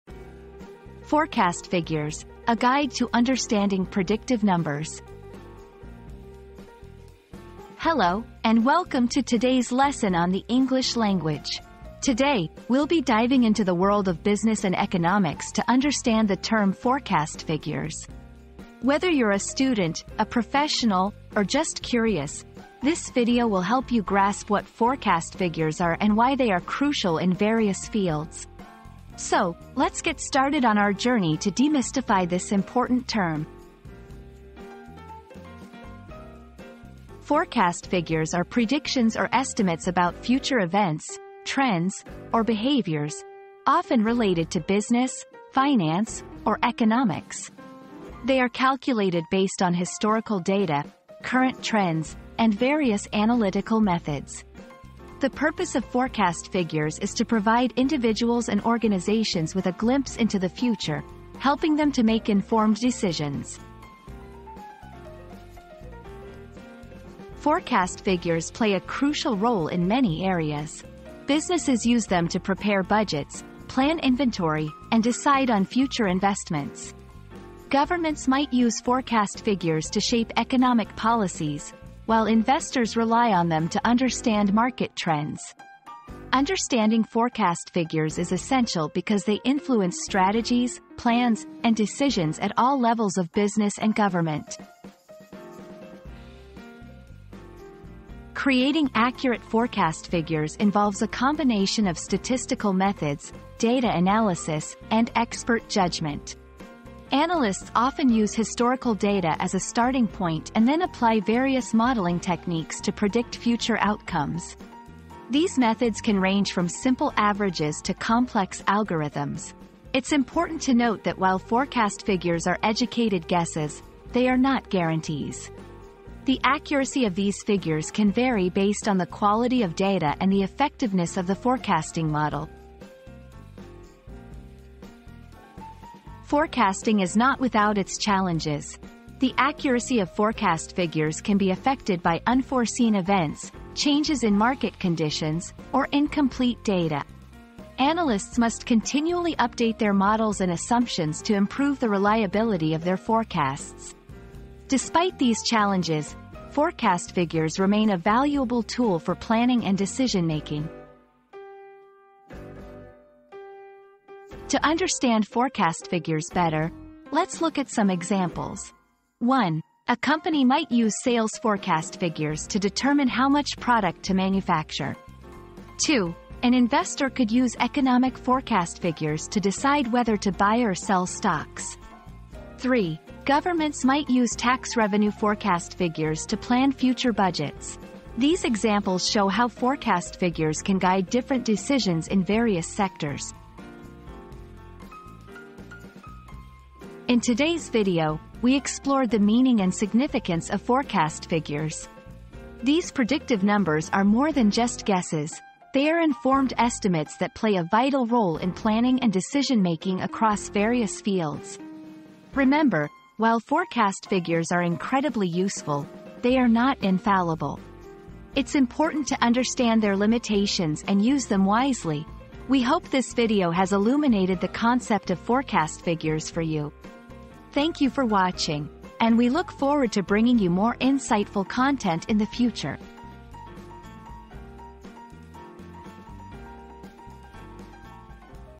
Forecast Figures: A Guide to Understanding Predictive Numbers
Decoding Forecast Figures: Your Guide to Understanding Predictive Numbers • Unlock the secrets behind forecast figures and gain a comprehensive understanding of predictive numbers. This enlightening guide will demystify complex data and equip you with the knowledge to make informed decisions based on accurate forecasts. Don't let numbers confuse you anymore, watch now!

00:00 • Introduction - Forecast Figures: A Guide to Understanding Predictive Numbers
00:36 • What Are Forecast Figures?
01:06 • The Importance of Forecast Figures
01:37 • How Are Forecast Figures Created?
02:15 • Challenges in Forecasting
02:43 • Examples in Real Life